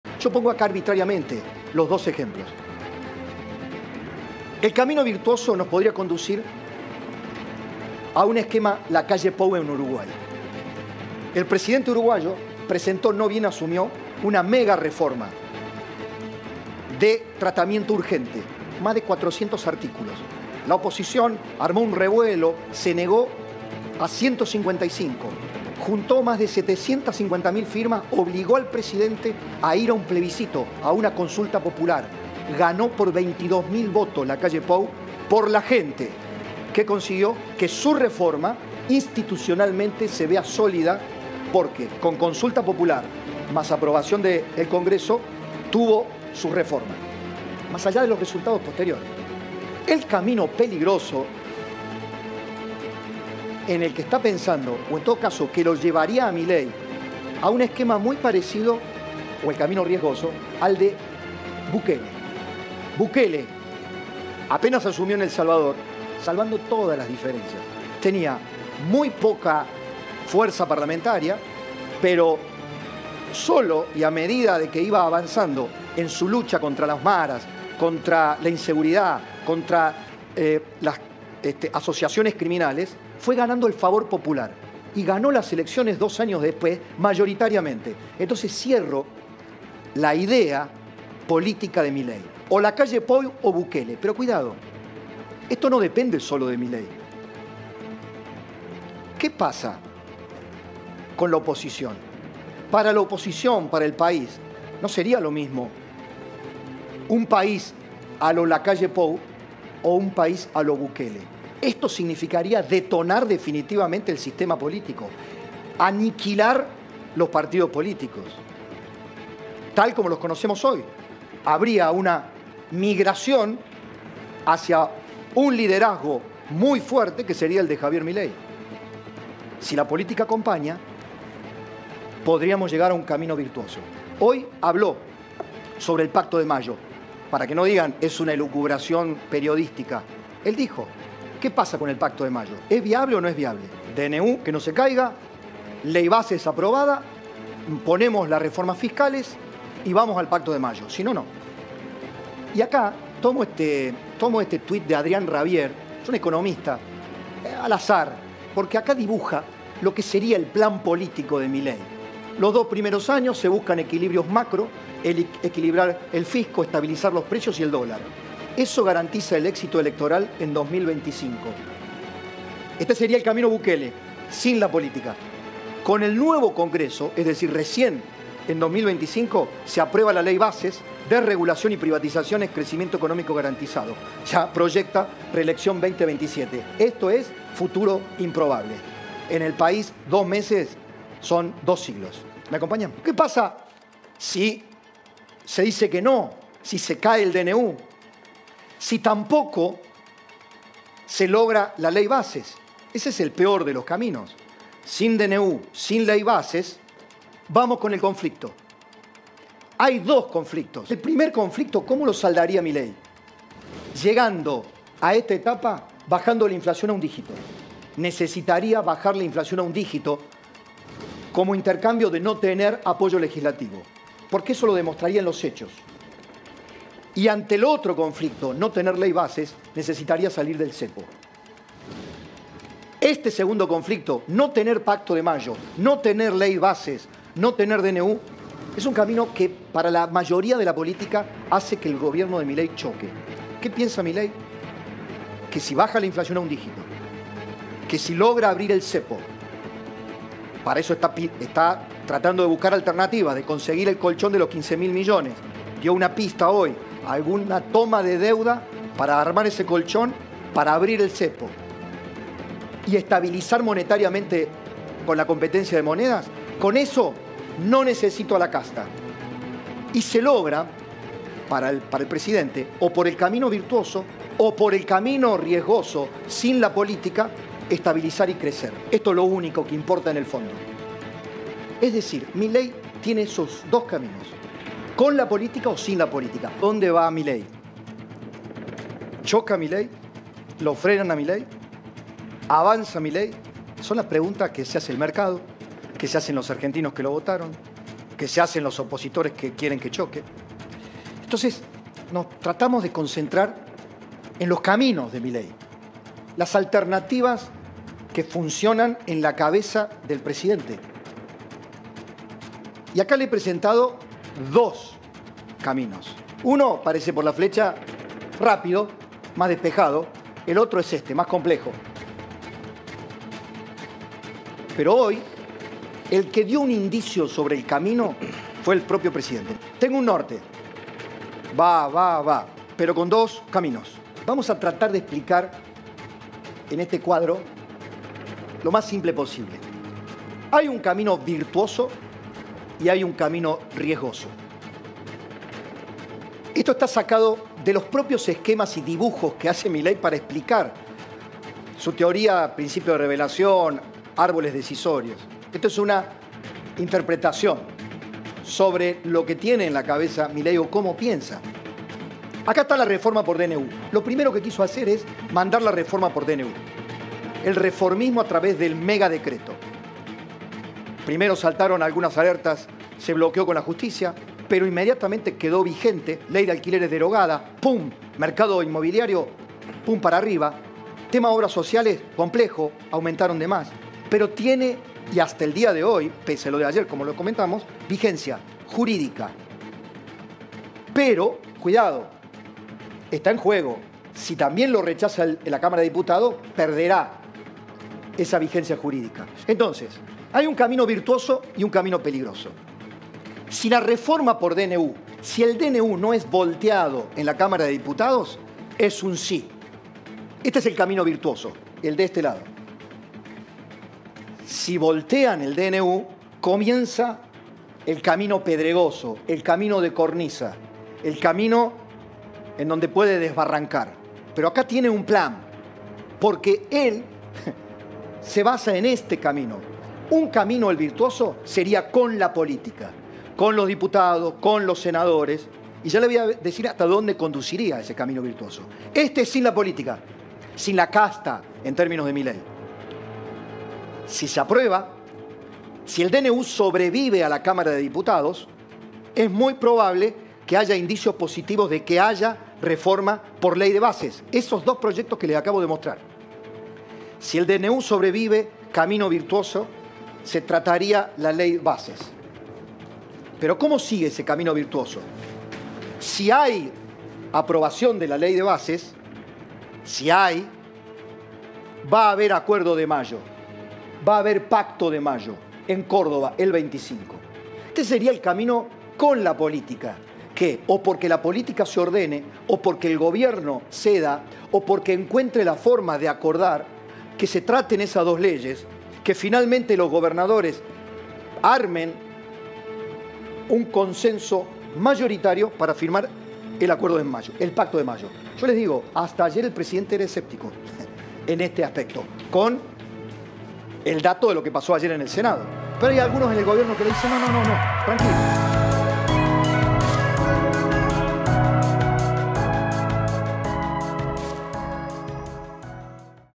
🇦🇷🤝🏻🇸🇻 | MILEI A LO BUKELE: El Camino Sin La Política.
Hay dos enfoques posibles: uno basado en la política, representado por la aprobación del DNU, la Ley Bases y el Pacto de Mayo, y otro que sigue un liderazgo más directo, como el ejemplificado por Nayib Bukele.